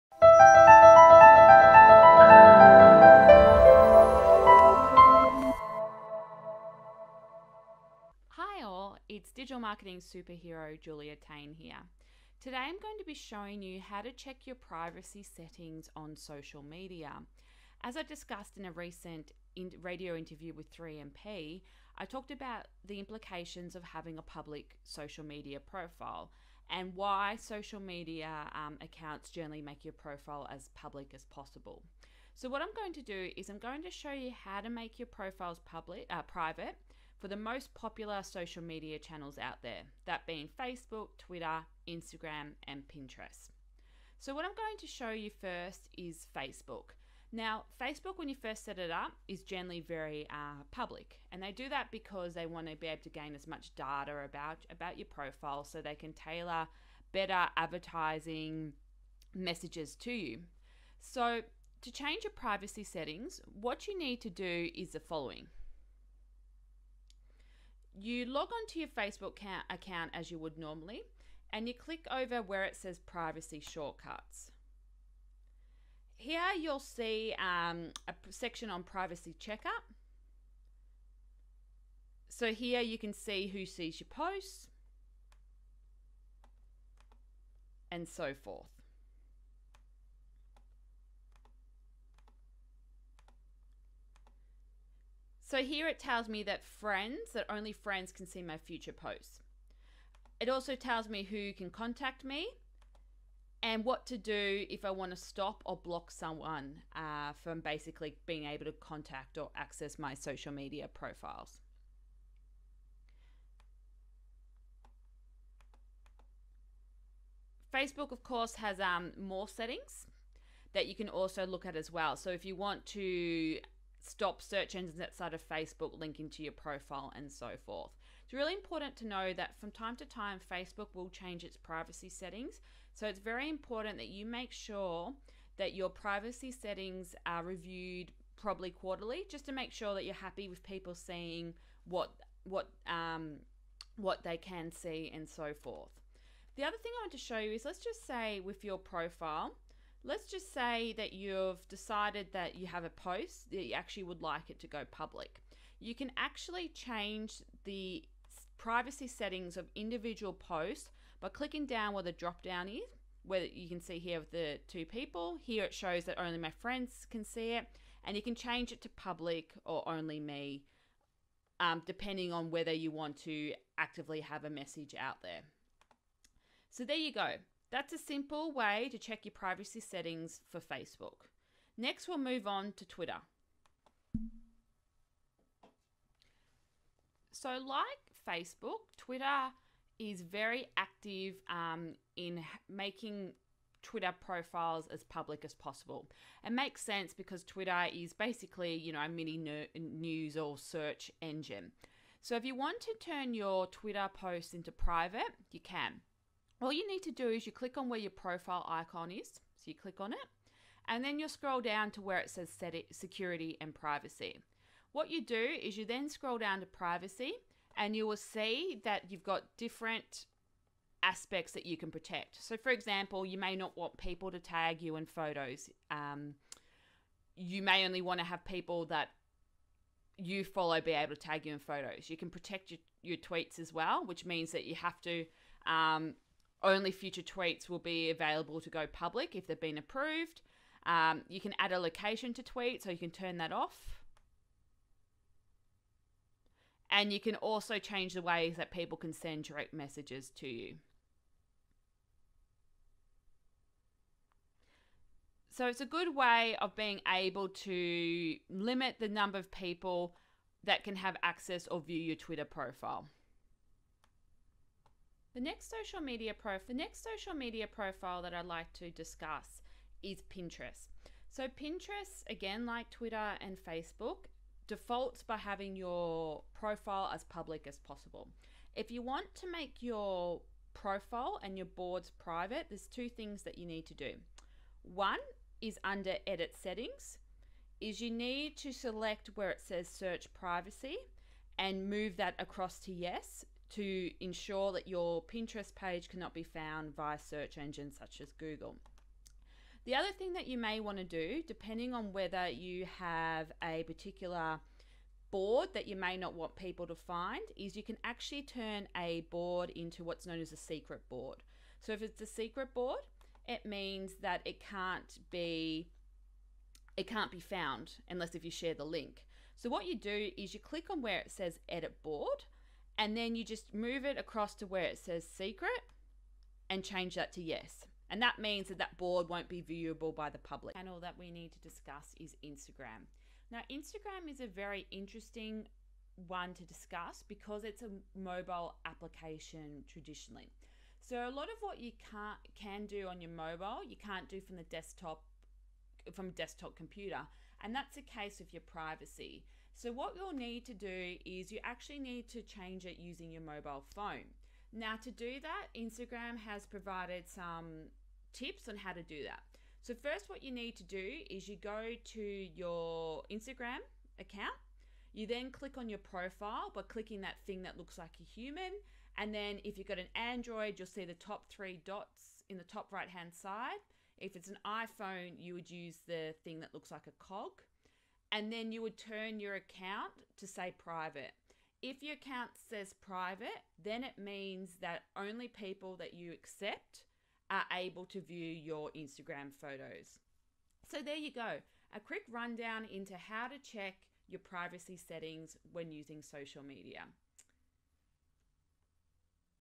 How to check your social media privacy settings
There’s no denying that the advent of the World Wide Web and in particular social media has changed the way we interact with businesses, friends and colleagues. At Vent2Me we love the digital landscape because for the first time small business and individuals have access to the same platforms, resources and tools as large businesses, industry leaders and celebrities.

But that’s not to say that the internet is not without its risks. So how do you ensure you stay safe online?

The default setting for most social media sites is to have your social media profile set to ‘public’. You can easily check the privacy settings to ensure only those you know see your content. To help, we’ve created a simple video to show you how to do this. It's important to check your privacy settings as they are prone to change.